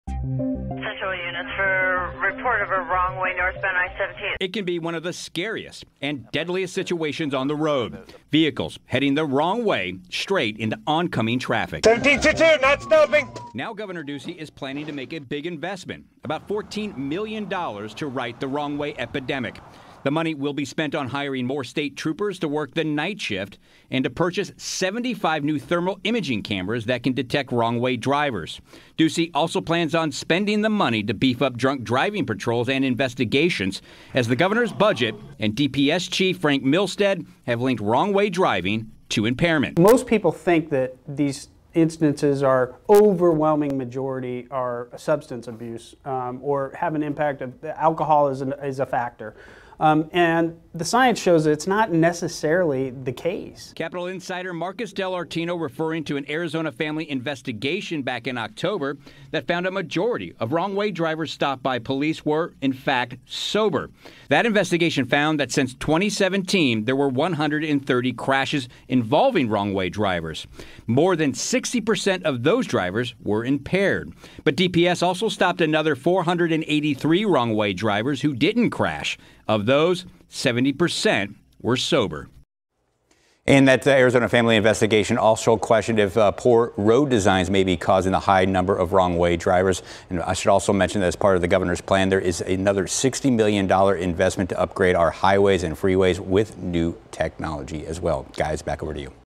Gov. Ducey's state spending plan includes effort to curb wrong way drivers
The Governor's budget plan will devote about 14 million dollars to prevent wrong way drivers. The plan places a heavy emphasis on curbing drunk driving as law enforcement officials believe it is a root cause of wrong way drivers.